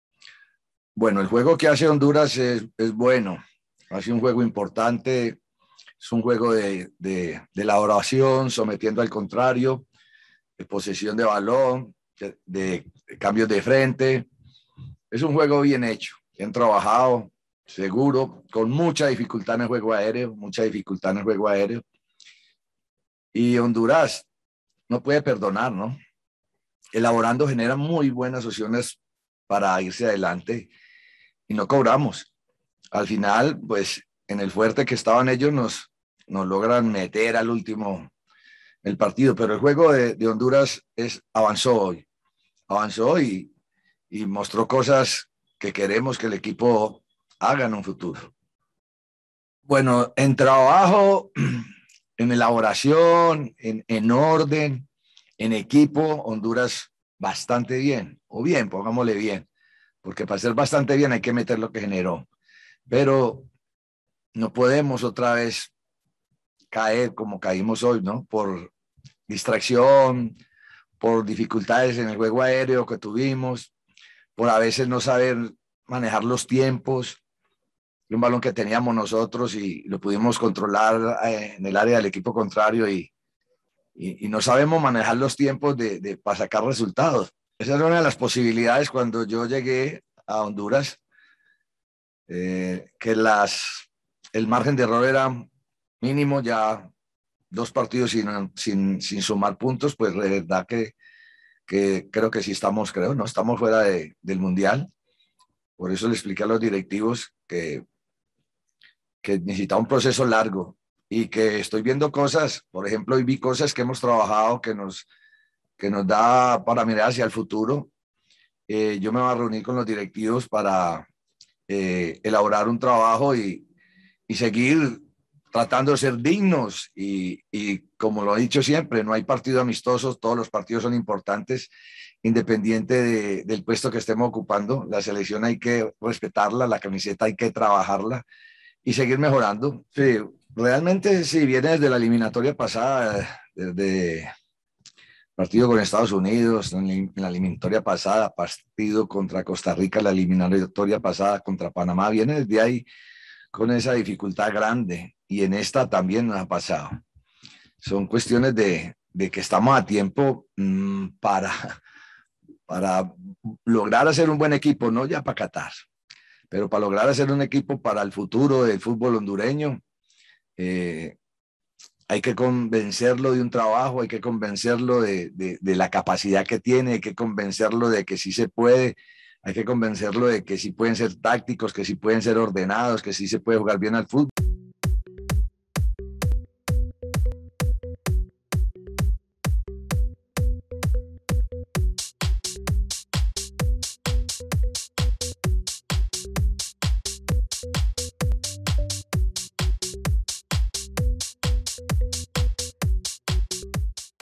Hernán Darío Gómez: "Creo que SÍ NOS HEMOS (Selección de Honduras) quedado fuera del Mundial"
🍀 Regístrate en 1xBet con este link y llévate un bono de hasta 130 euros en tu primer depósito